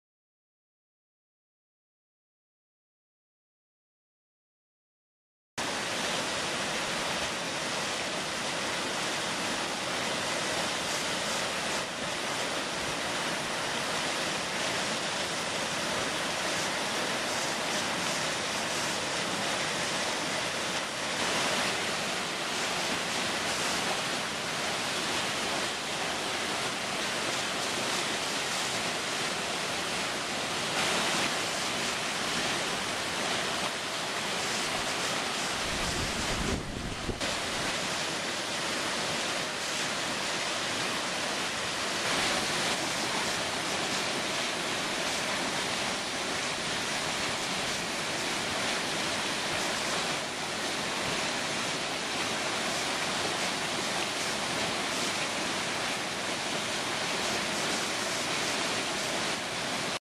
River Surf, vectors of whitewater
Kayak surfing on the South Fork of the American River. Kayak stays in the surf wave because it is sliding down the wave faster than the speed of the current. Some of my favorite tools